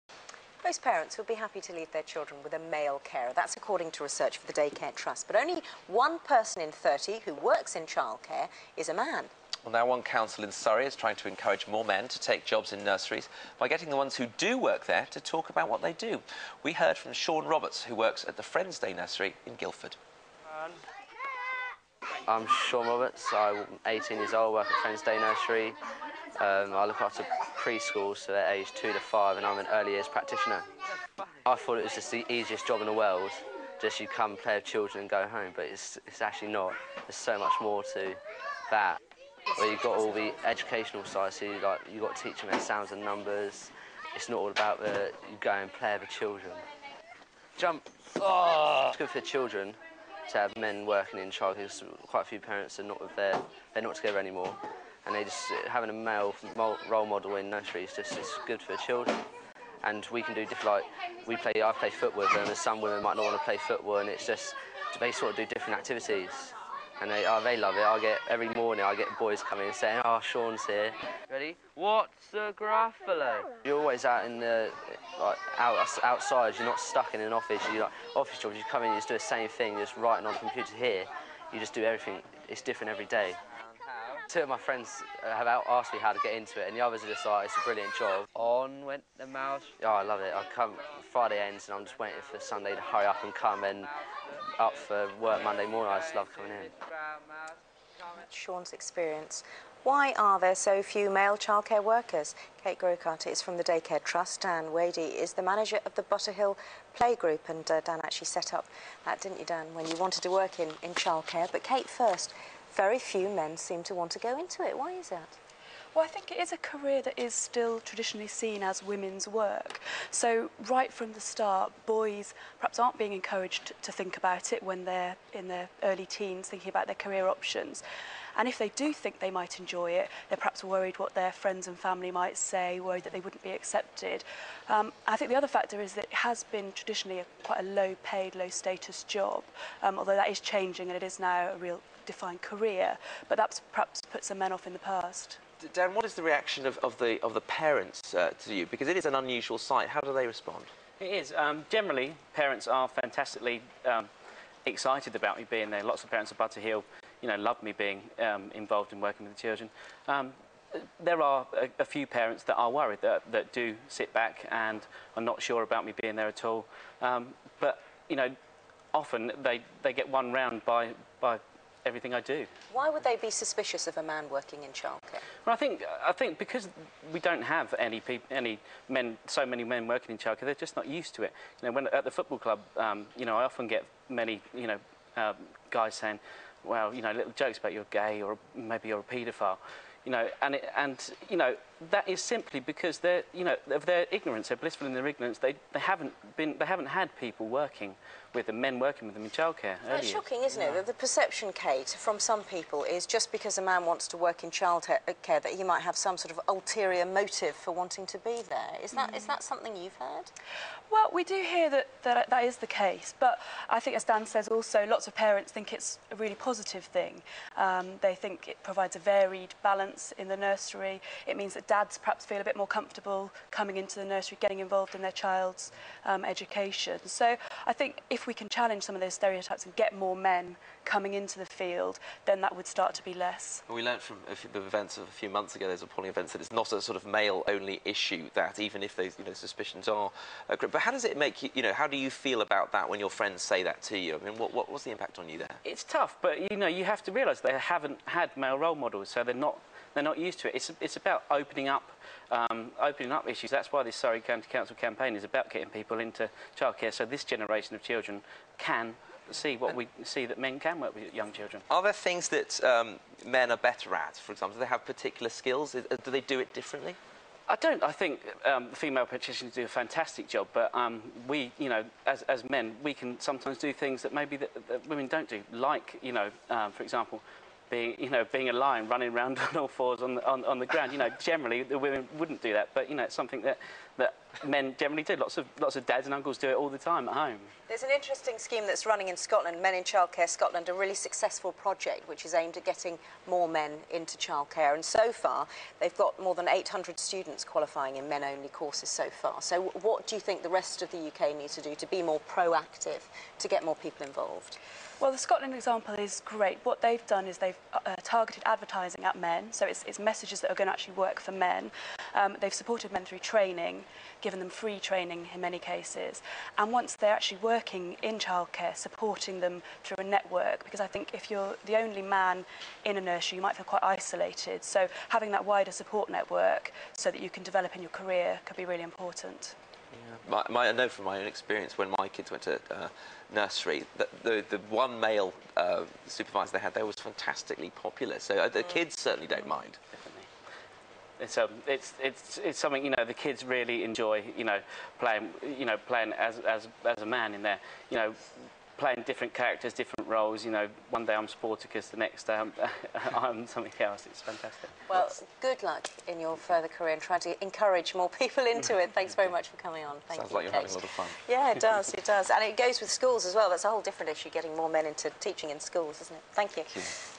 Get More Men into Childcare?
Only 1 in 30 childcare workers is male. So a campaign has been launched to get more men working with children.